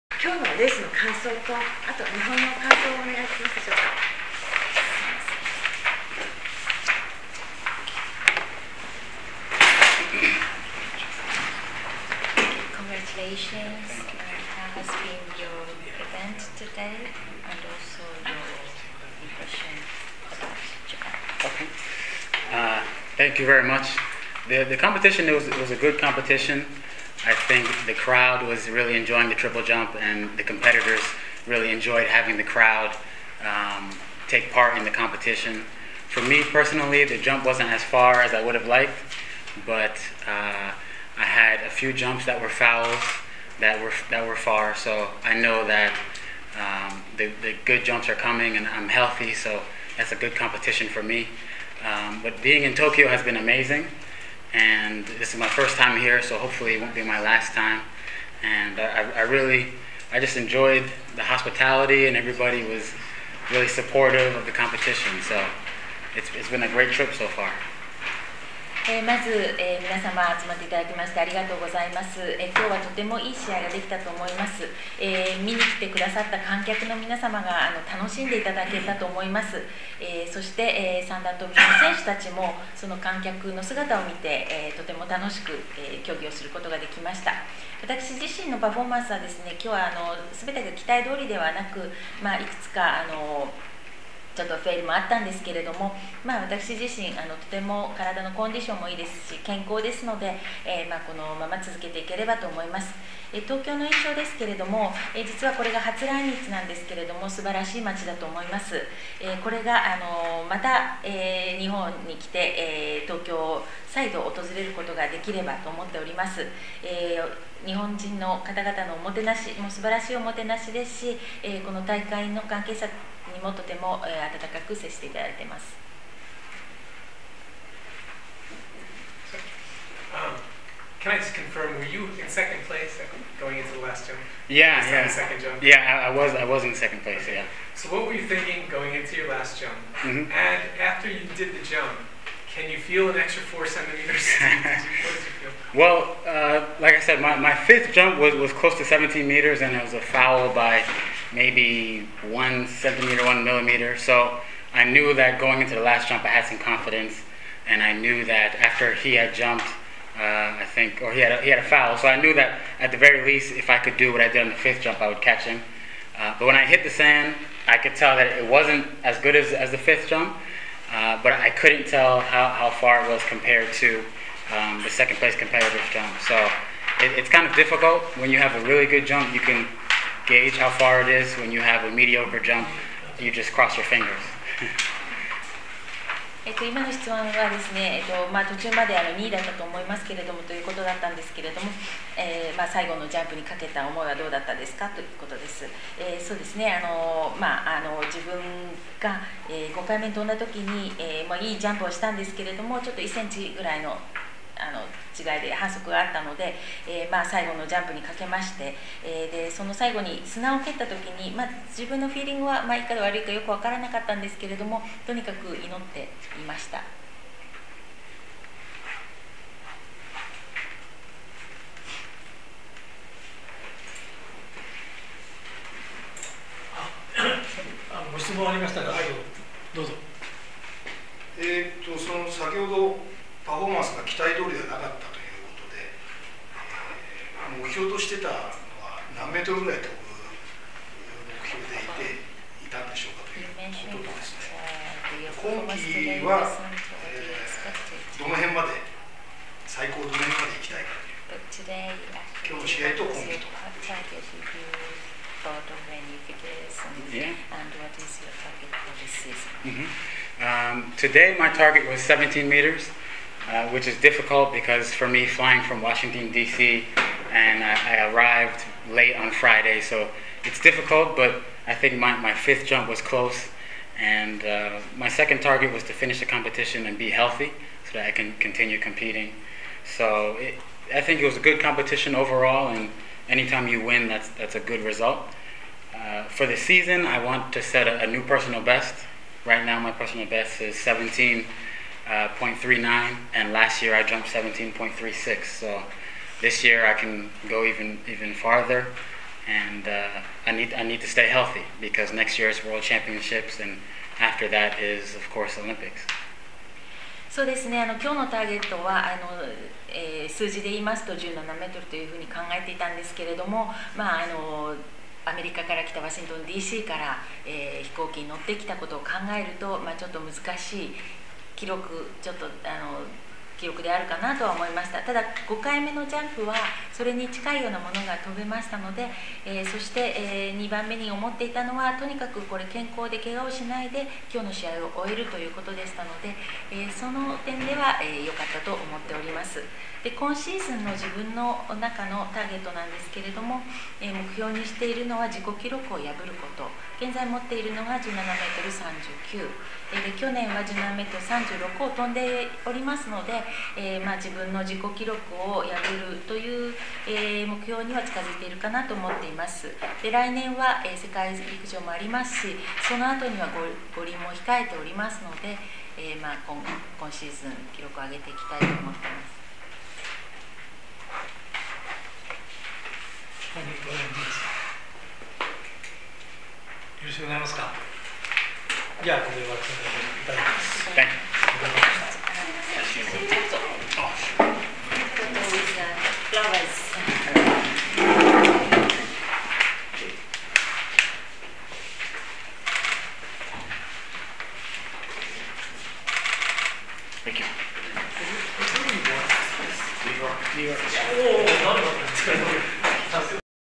Goldengrandprix Men Triple Jump LAINE, Samyr（HAI）
SEIKO GOLDEN GRAND PRIX 2014 TOKYO
Sunday, 11 May, 2014
National Stadium (Kasumigaoka)